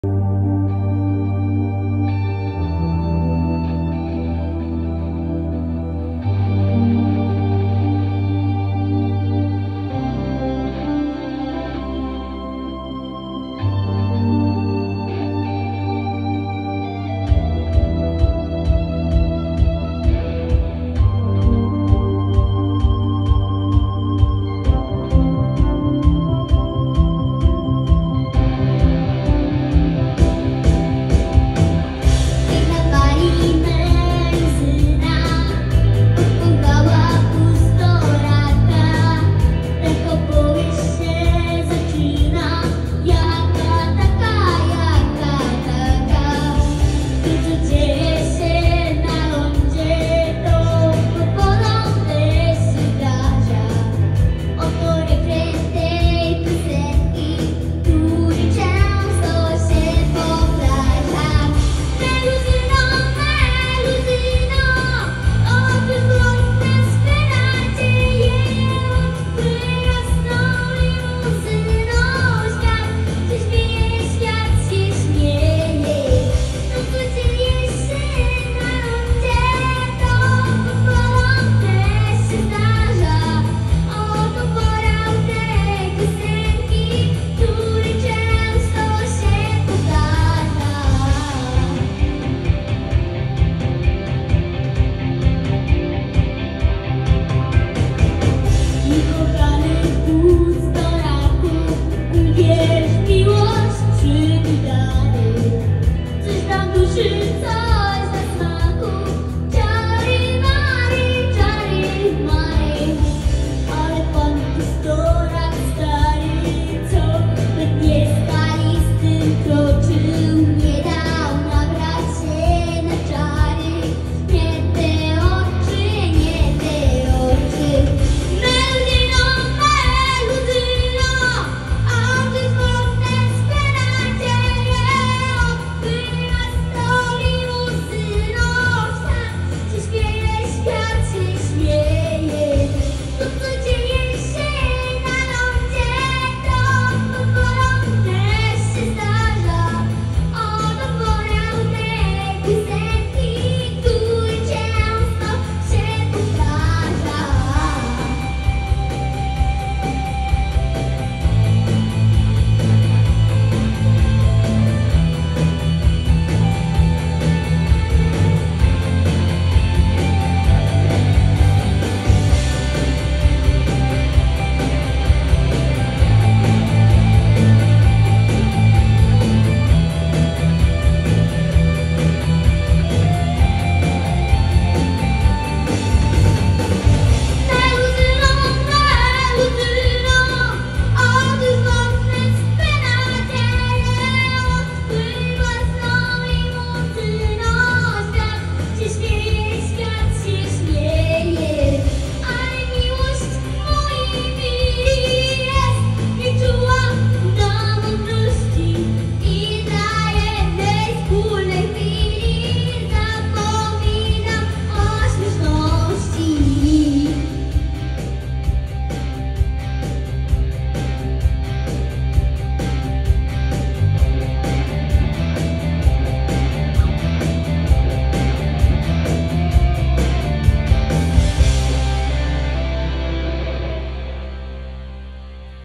XI MINI TALENT SHOW - Maja Nowak
Cześć ,
nazywam się  Maja Nowak mam 9 lat.  Chodzę do 2 klasy w ZPO Kasinka Mała. Lubię śpiewać , tańczyć i rysować .
Zapraszam wszystkich do wysłuchania tego co przygotowałam 😉